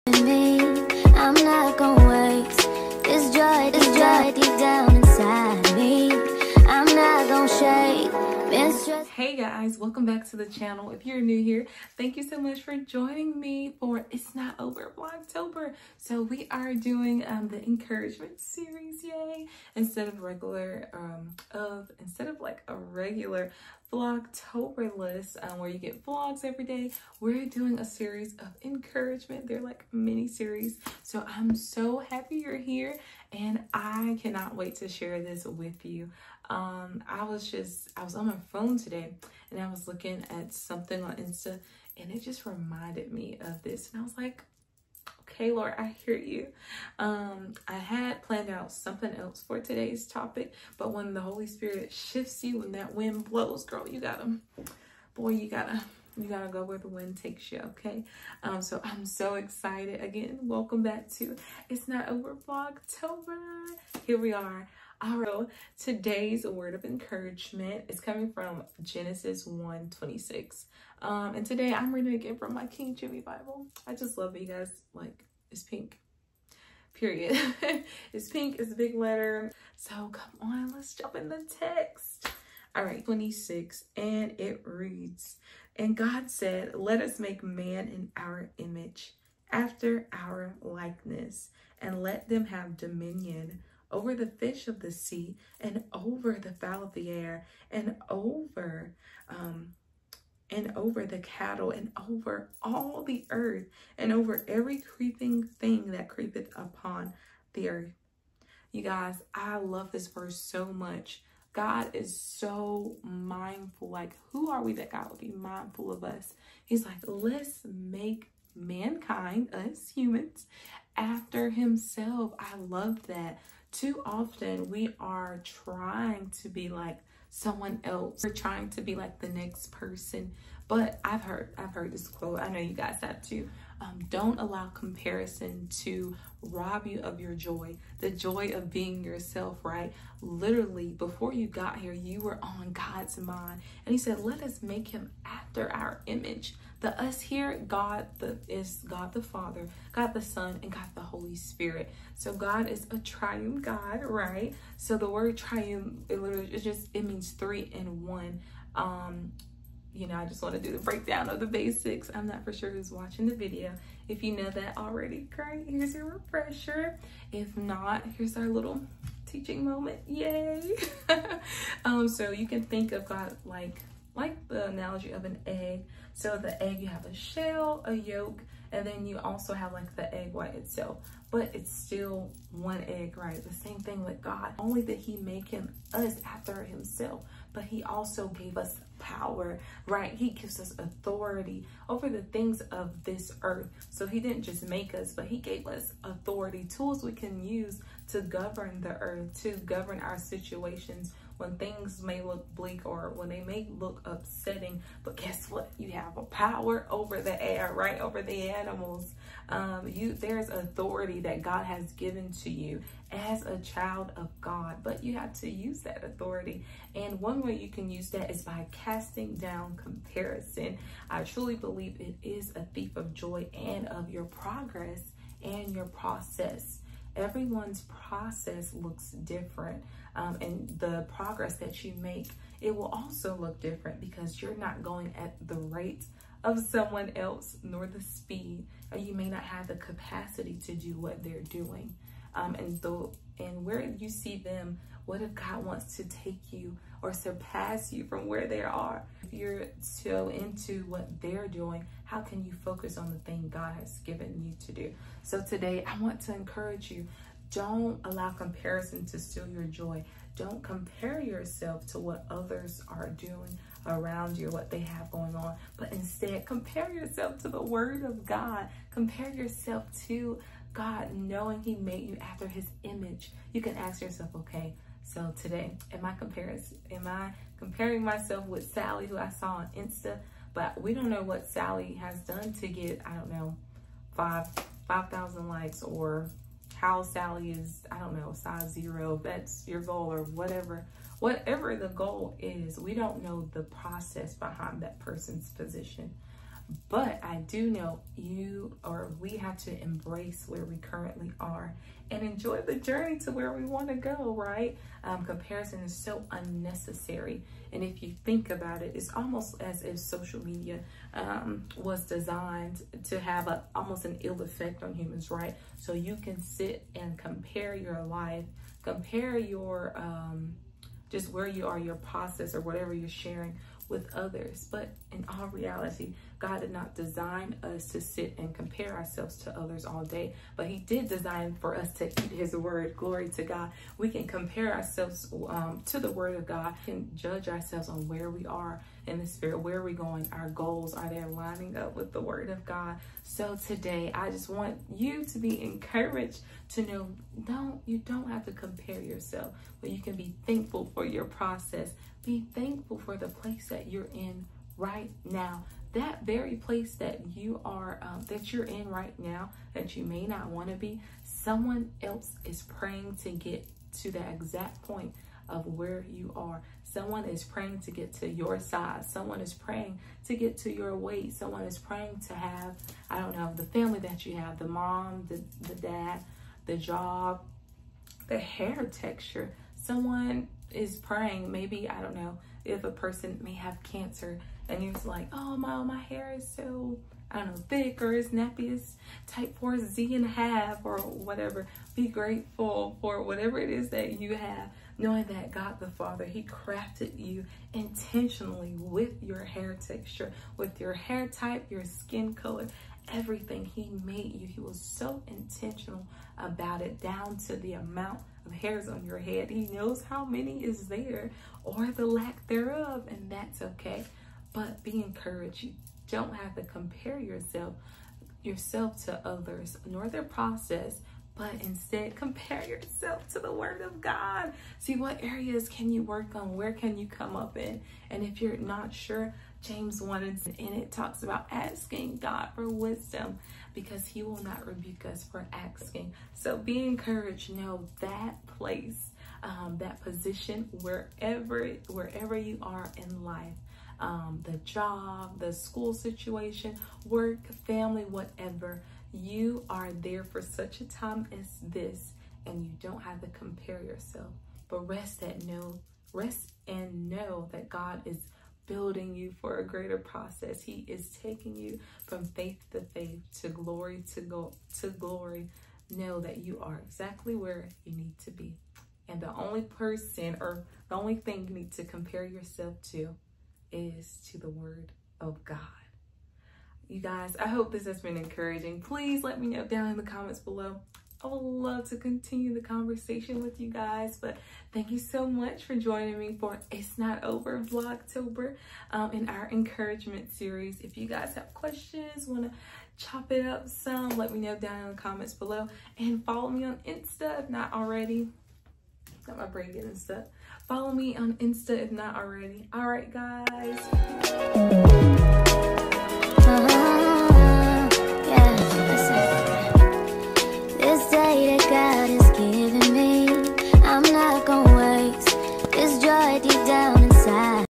Comparison l It’s Not Over | Vlogtober | Day 3
✨Happy Vlogtober !

🍂Have you noticed yourself scrolling and discouraged after stepping away from your phone? Comparison robs us of our job and the makes us forget about our achievements! Let’s dive in the Word together as we work to eliminate the feelings of comparison!

✨These Encouragement Series is designed to inspire you to Finish this year strong! 💪🏾

🍂When Life weighs you down, one things for sure, God’s Word will always Lift you up!

💛Personal IG:
💛Business IG: blessed_creations_xo
💛FB Business Page:
💛Blessed Creations Art Studios

🦋Enjoyed this Video?!